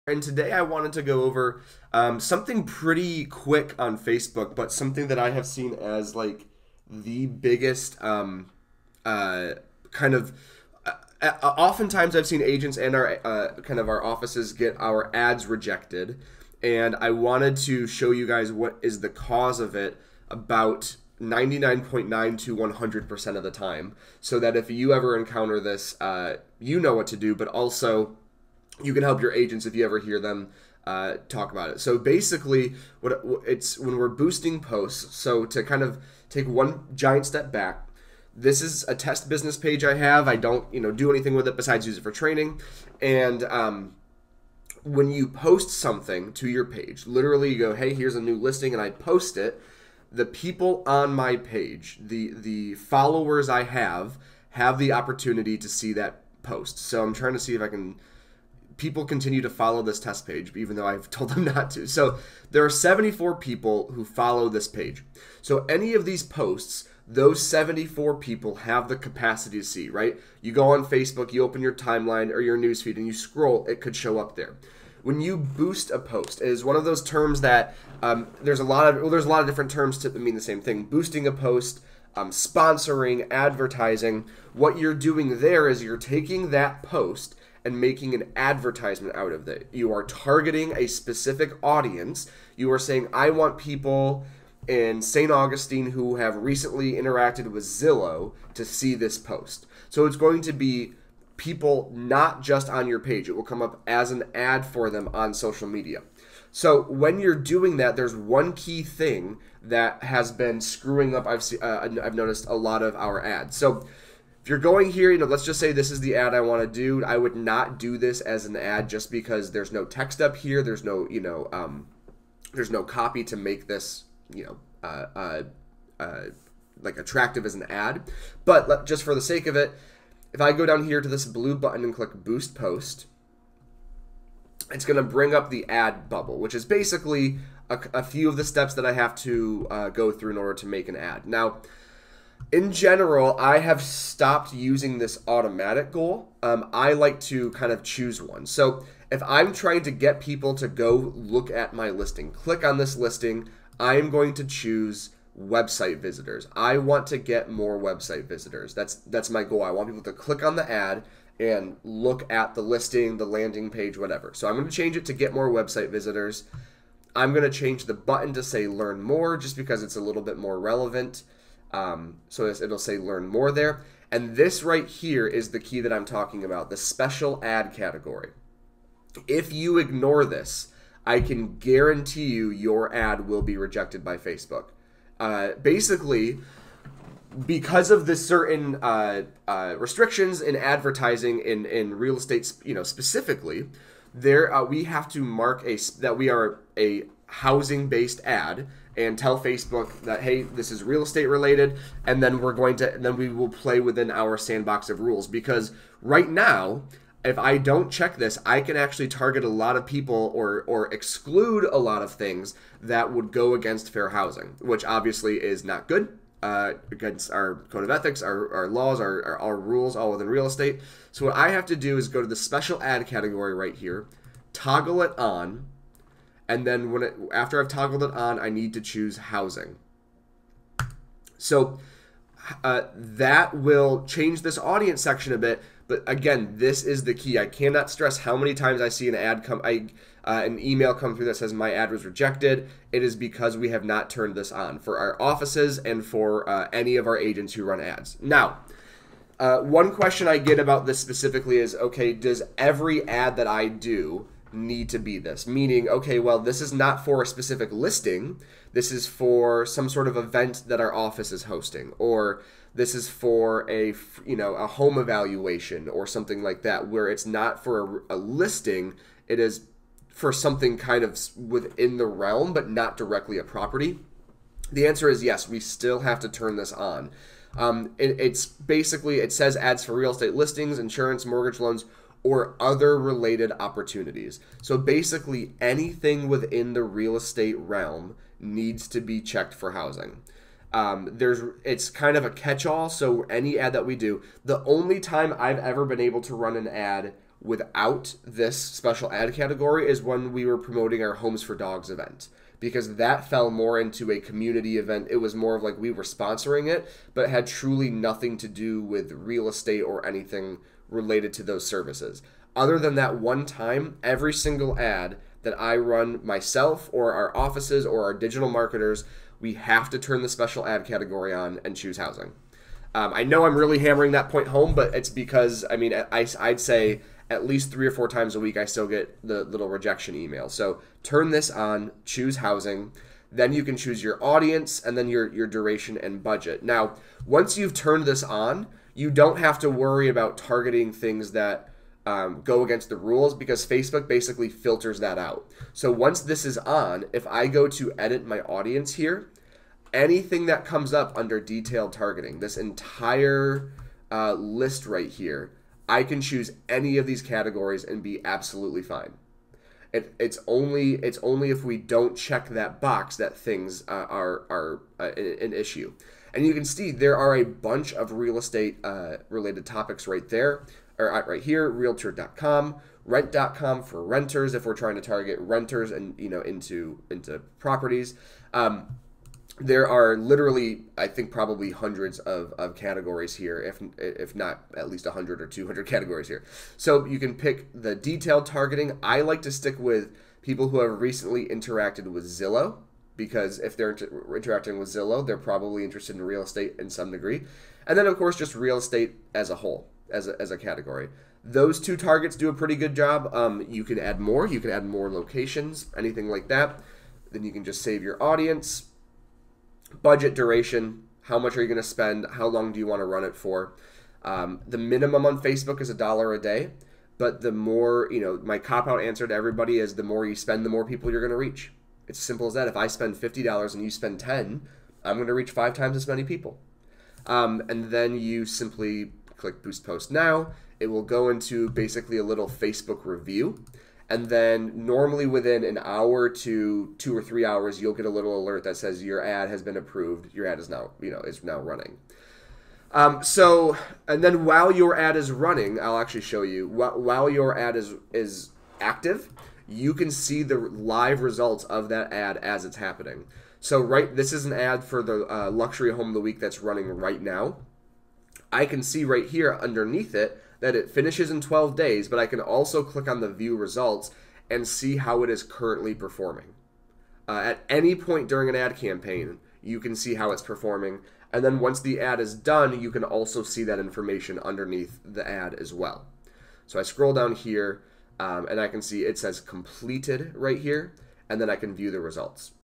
FB Special Ad Category - BlueSync, June 23
Today we review THE MOST IMPORTANT PART of boosting an ad on Facebook. 99.9% of the time your ad is rejected, it's because you missed this one crucial step!